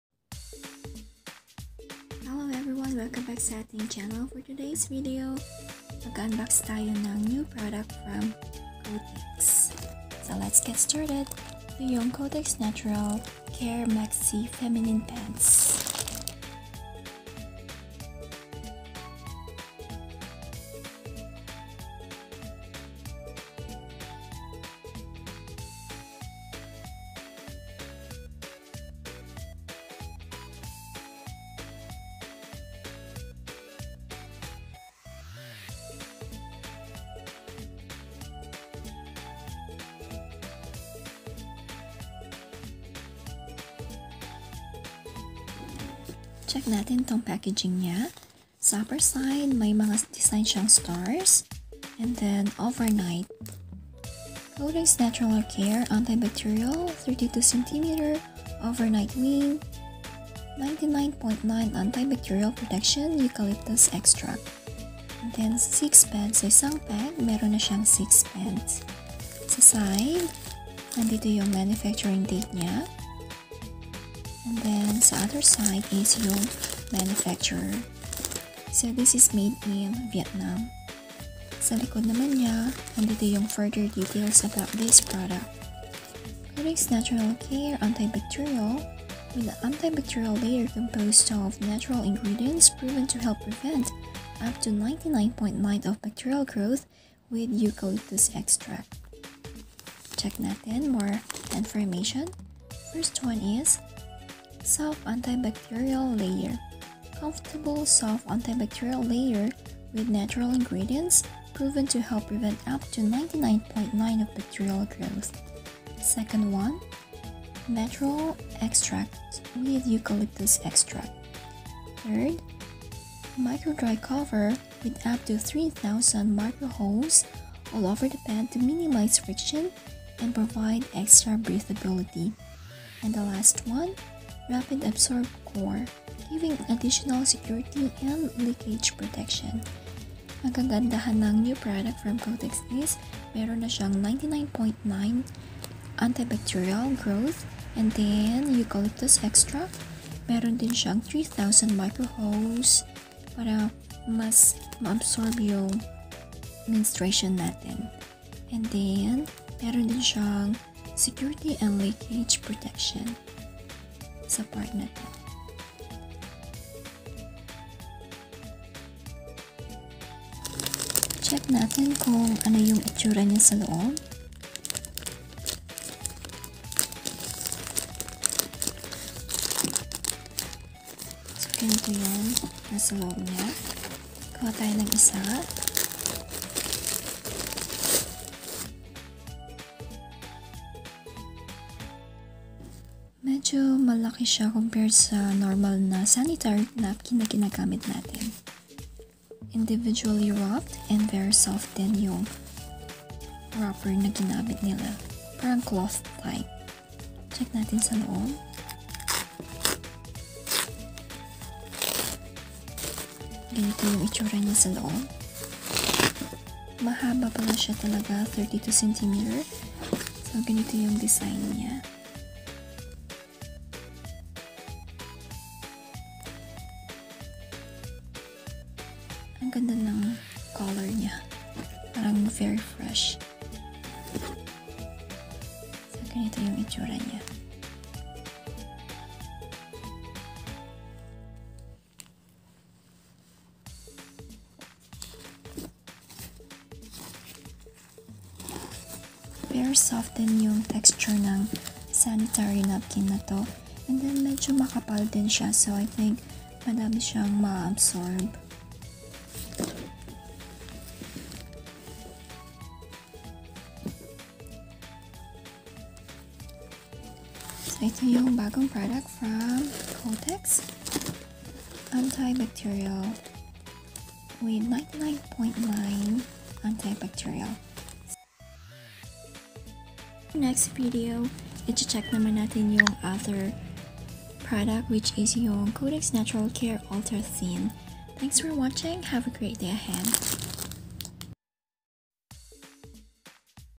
Kotex Anti-Bacterial Protection [Overnight Maxi Feminine Pads Review]
Hello everyone!

If you're  looking for an extra care down there, Kotex Natural Care Anti-bacterial feminine pads must be considered. This is new in the Philippine market and promises a better protection among women during their period .

Kotex Natural Care Anti-bacterial. With an anti-bacterial layer composed of natural ingredients proven to help prevent up to 99.9% of bacterial growth, with Eucalyptus extract.

The new Kotex anti-bacterial sanitary napkin has soft anti-bacterial layer. Comfortable soft anti-bacterial layer with natural ingredients proven to help prevent up to 99. 9% of bacterial growth.

It has the following features;

1. Natural Extract
With eucalyptus extract;

2. Micro-dry Cover
With up to 3000 microholes all over the pad to minimize friction and provide extra breathability.

3. Rapid Absorb Core
Giving you additional security and leakage protection.

Try Kotex Natural Care Anti-Bacterial feminine pads to experience comfort and safety during your red days.

Thanks!